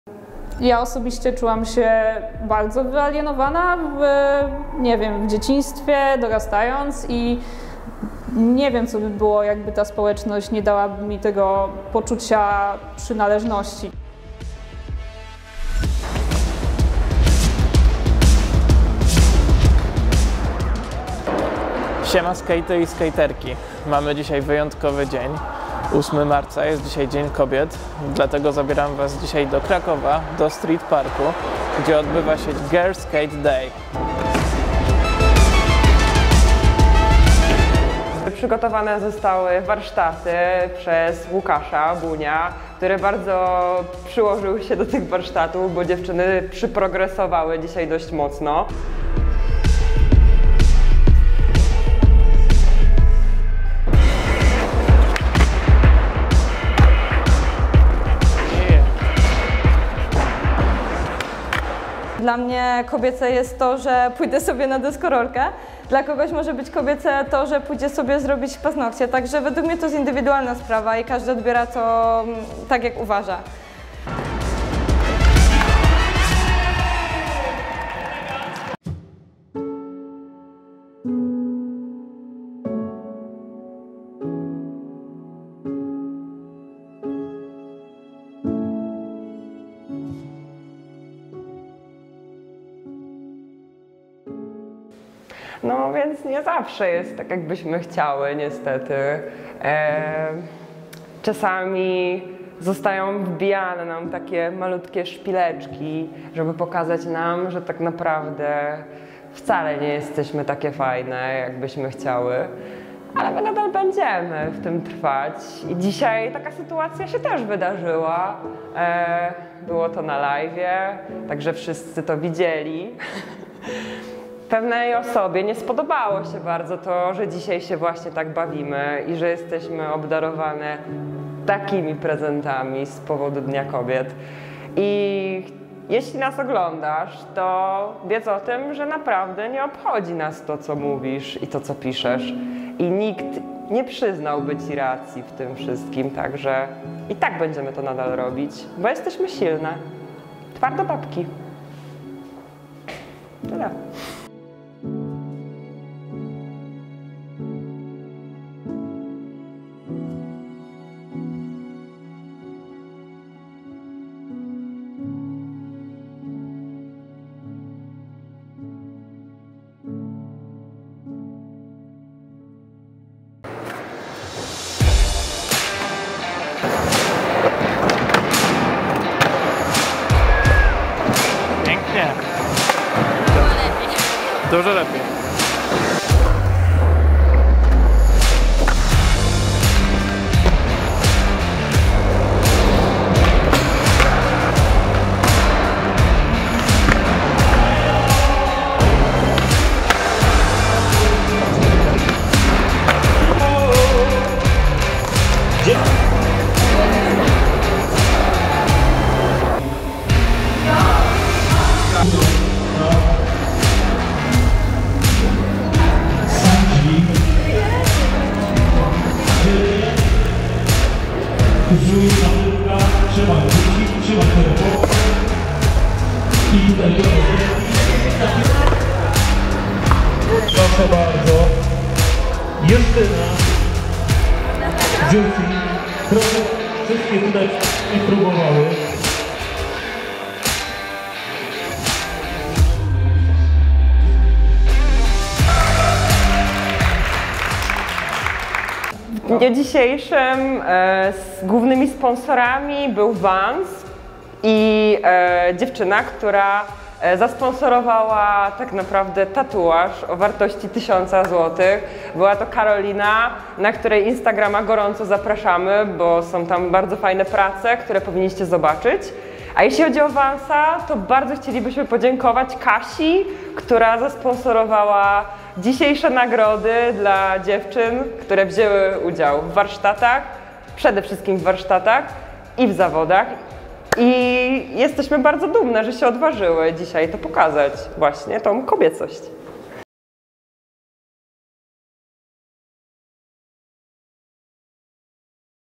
Dziewczyny na deskorolkach "Kobiece jest to, że pójdę na deskorolkę" | SKATE STORY #20
Na miejscu dla skejterek czekały wystawy od dziewczyn związanych z deskorolkową sceną. Były specjalnie przygotowane obrazy i różnego rodzaju fotografie. Oprócz tego dla wszystkich dziewczyn został zorganizowany Jam Deskorolkowy, który w pięknym stylu wygrała Ania Kulig. Dziękujemy za kozacką imprezę!